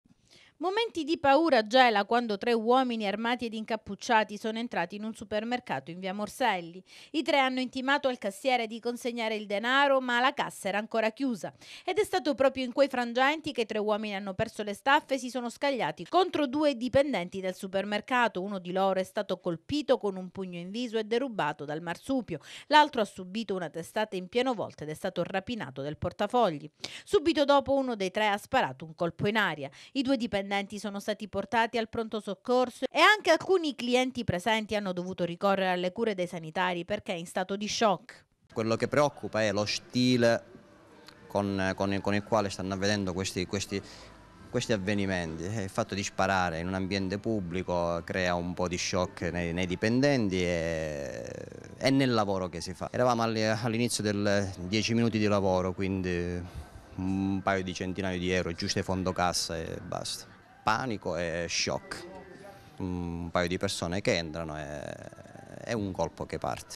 GELA, RAPINA AL SUPERMERCATO
GELA, RAPINA AD UN SUPERMERCATO AGGREDITI PROPRIETARIO E IMPIEGATO